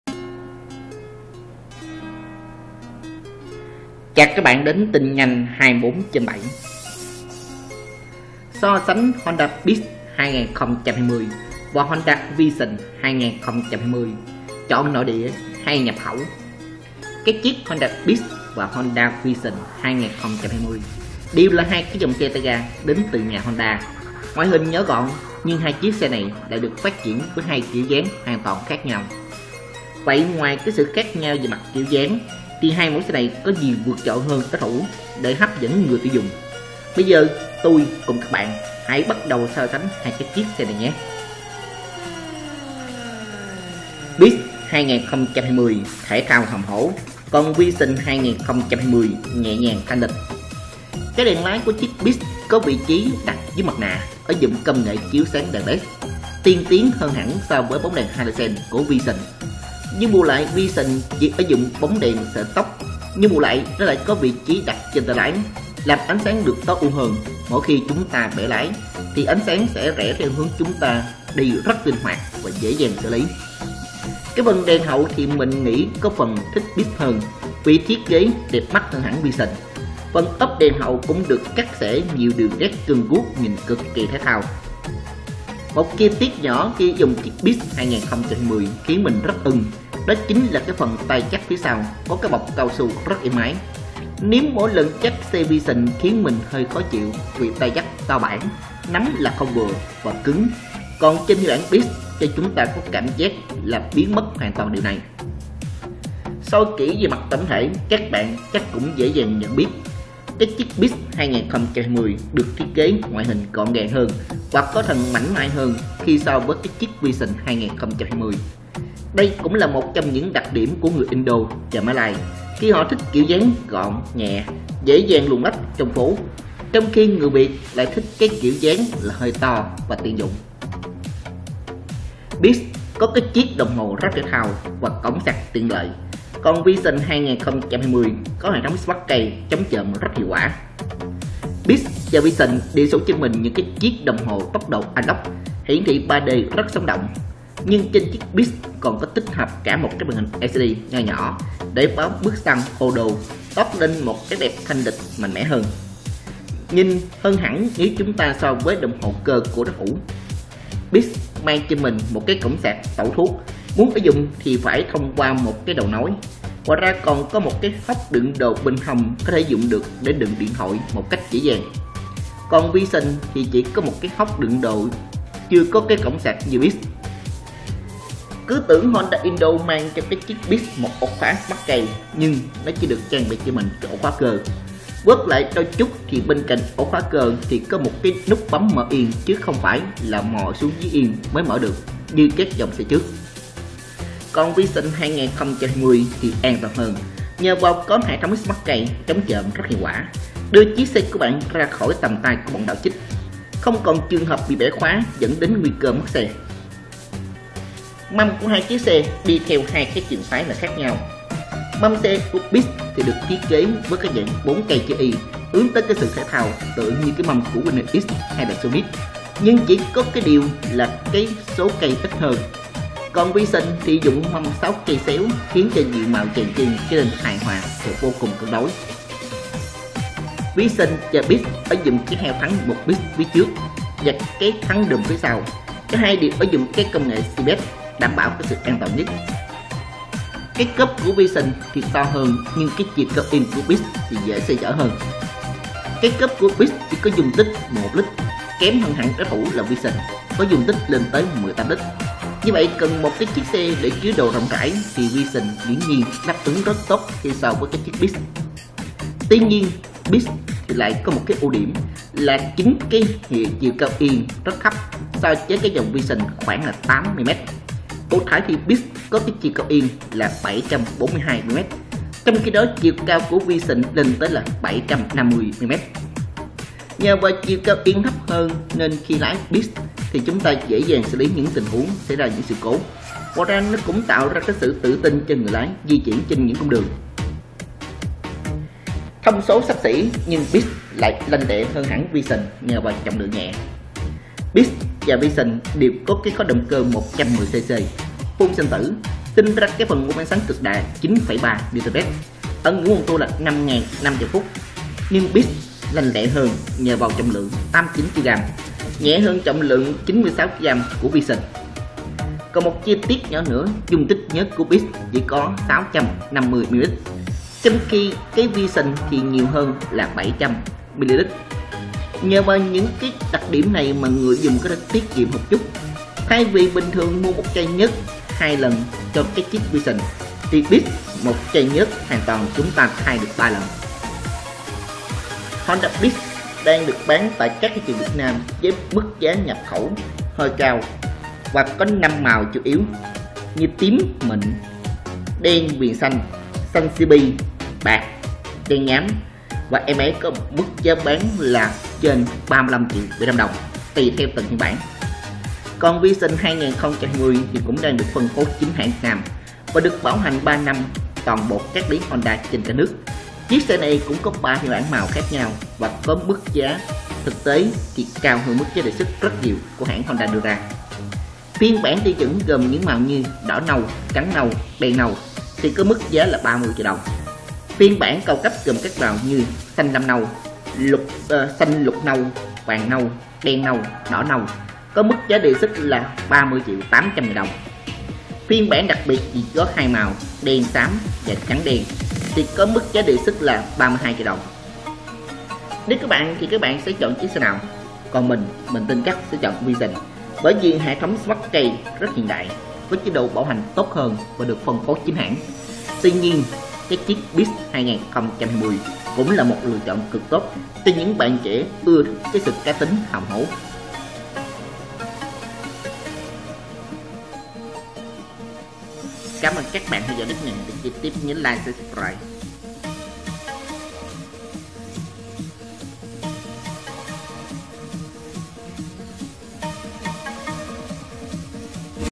Tin nhanh 24/7 - So sánh Honda Beat 2020 và Vision 2020 chọn nội địa hay nhập khẩu.
Honda Beat và Vision 2020 đều là hai mẫu xe tay ga đến từ nhà Honda có ngoại hình nhỏ gọn nhưng hai chiếc xe này lại được phát triển với hai kiểu dáng hoàn toàn khác nhau. Vậy ngoài khác kiểu dáng ra thì hai mẫu xe này có gì vượt trội hơn đối thủ để hấp dẫn người tiêu dùng? Bây giờ mình và các bạn sẽ cùng nhau bắt tay vào so sánh hai chiếc xe này.